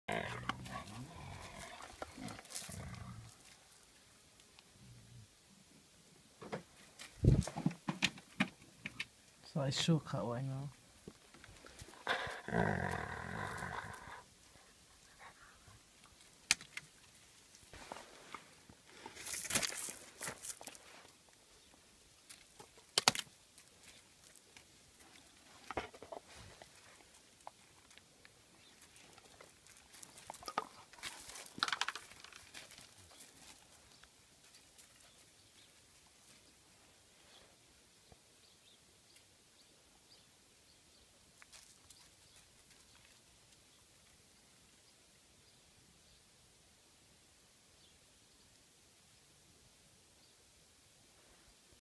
RIP Volli ♥️ Hungarian Vizsla 15 Years Old
Hungarian vizsla living in Spain. She was 15 years old when she passed